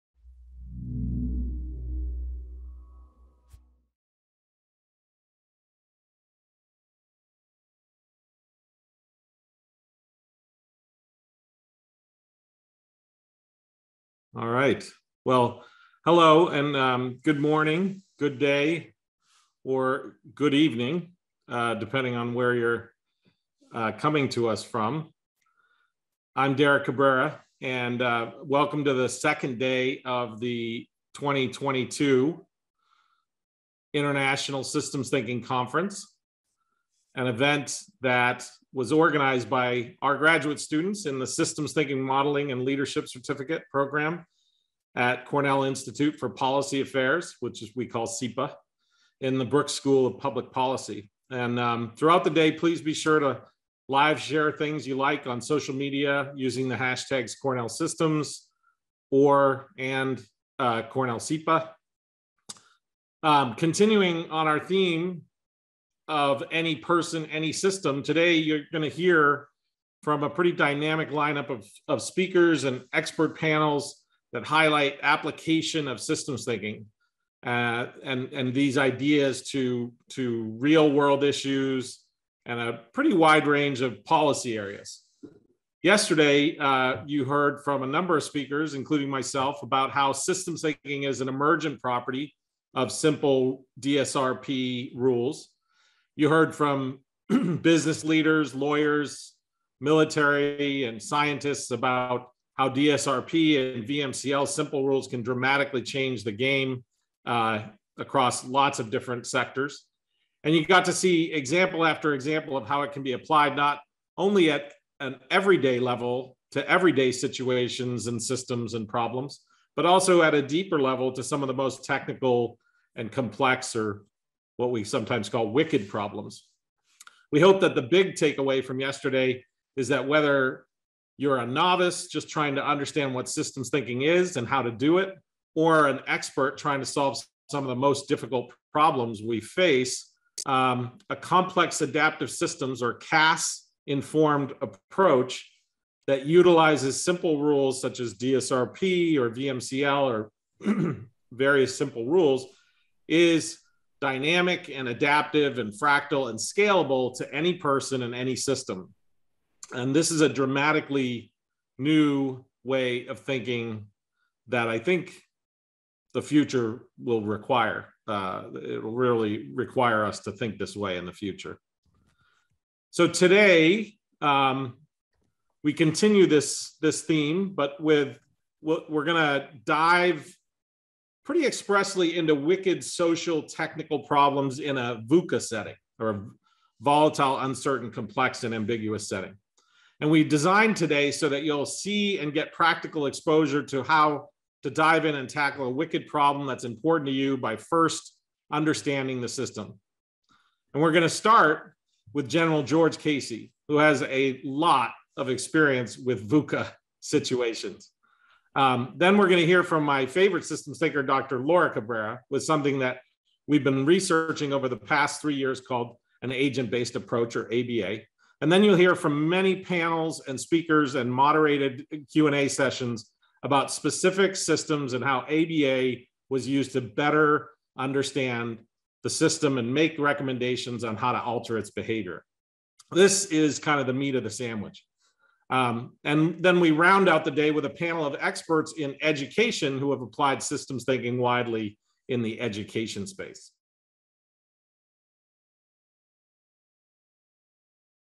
Dr. Derek Cabrera kicks off the second day of the 2022 conference | Academic Conferences
Dr. Derek Cabrera kicks off the second day of the 2022 Cornell University International Systems Thinking Conference!